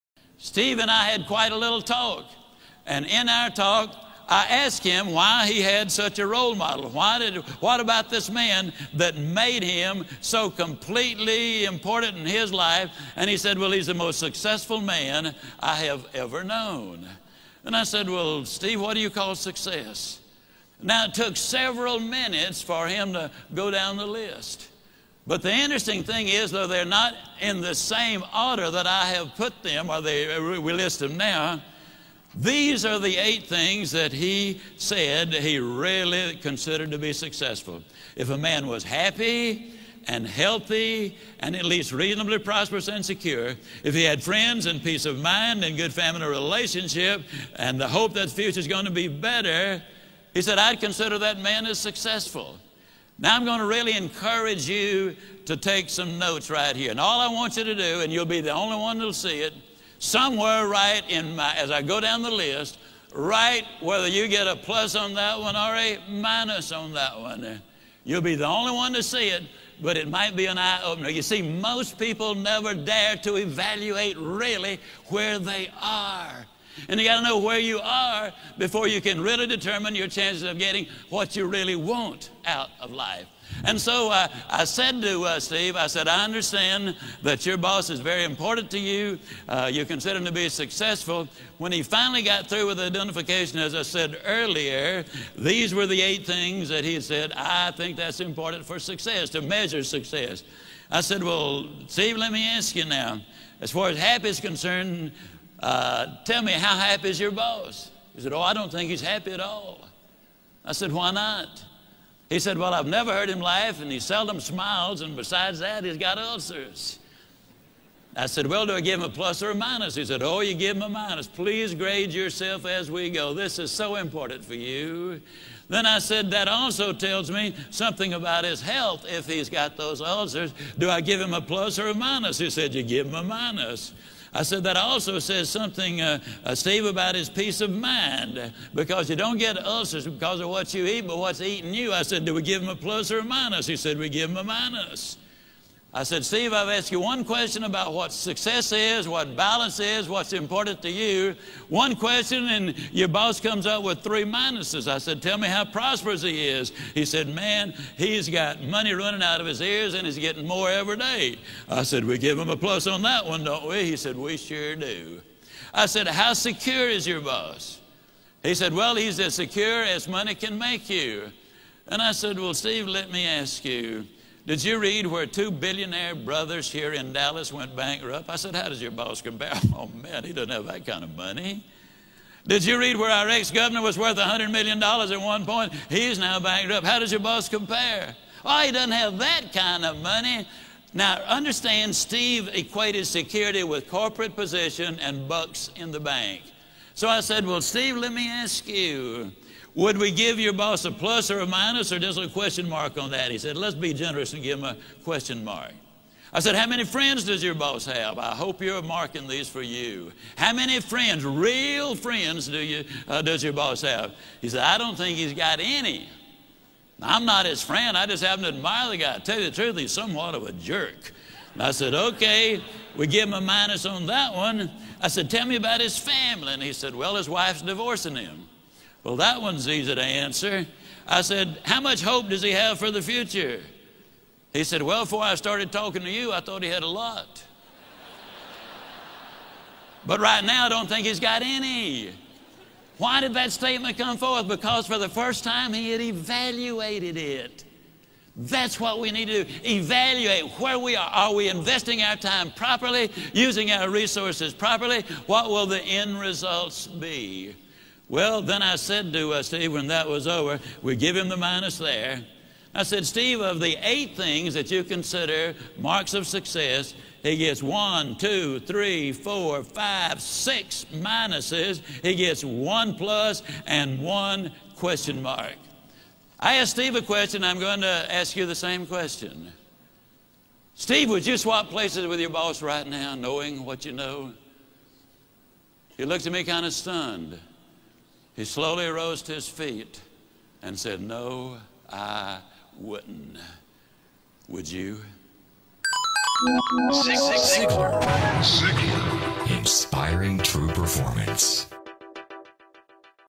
The Secret You Didn't Know | Zig Ziglar
zigziglar zigziglarmotivation zigziglarquotes In this Zig Ziglar's success formula motivational video ,Zig Ziglar defines what will make you know if you are successful or not at something you are doing . No need to stress yourself about whether you're on the right path to success or not just sit down and watch this video to clear every doubt about success.

Credit -YouTube :Zig Ziglar Inc.

 Recommended videos ;